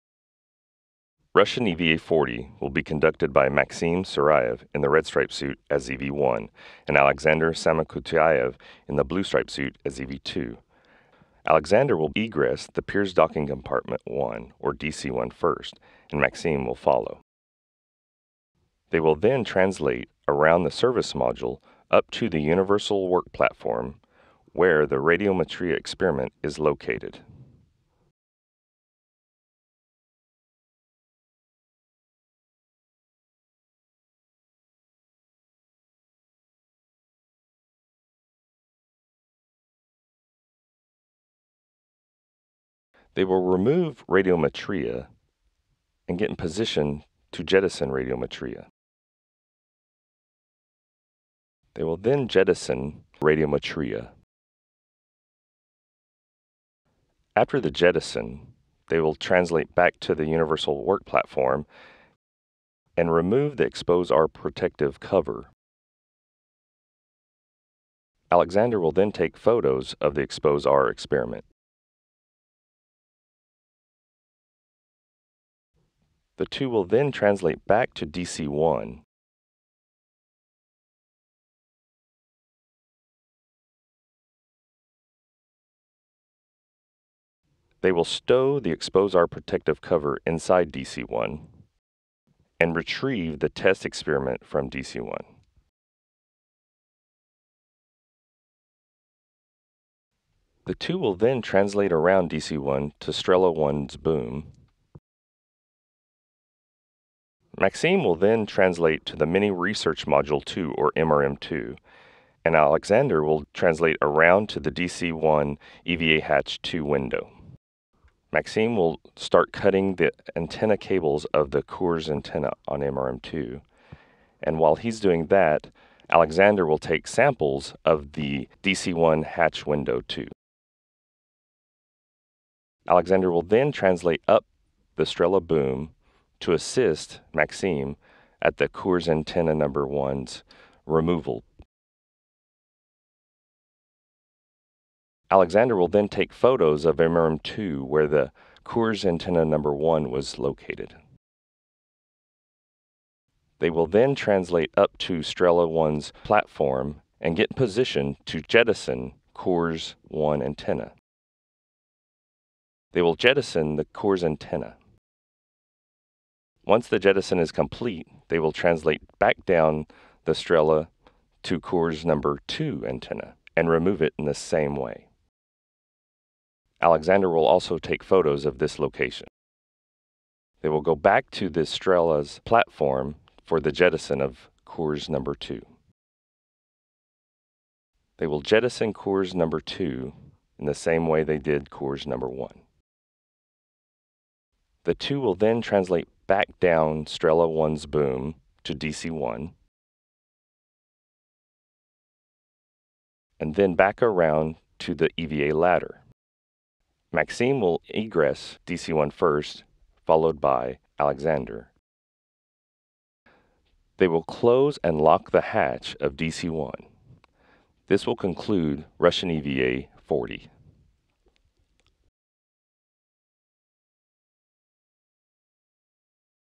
Tasks for Third Spacewalk of Expedition 41
NASA Spacewalk Specialist Devan Bolch narrates this animation depicting the events scheduled for the Oct.22 spacewalk. Expedition 41 Commander Max Suraev and Flight Engineer Alexander Samokutyaev of the Russian Federal Space Agency (Roscosmos) will remove and jettison experiment hardware and antennas no longer needed on the Russian segment of the complex and perform a detailed photographic survey of the exterior of the Russian modules. It will be the third spacewalk in as many weeks for Expedition 41 crew members on the station, the 184th in support of space station assembly and maintenance, and the second for both Suraev and Samokutyaev.